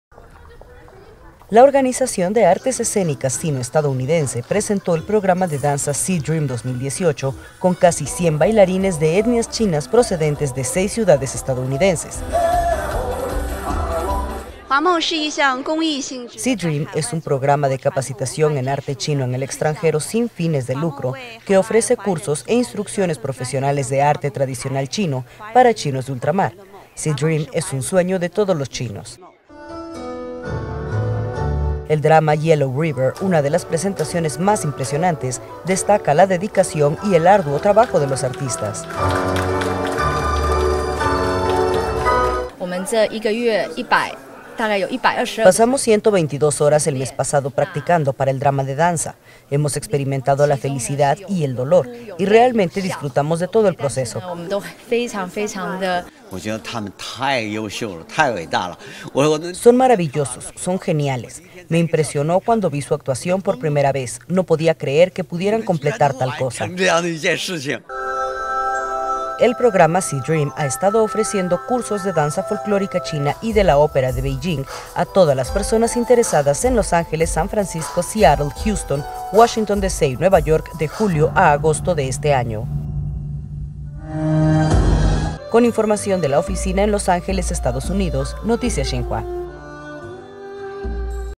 Evento de danza ‘C Dream 2018’ llega a Los Ángeles
La Organización de Artes Escénicas Sino-Estadounidense presentó el programa de danza ‘C Dream 2018’ con casi 100 bailarines de etnias chinas procedentes de seis ciudades estadounidenses.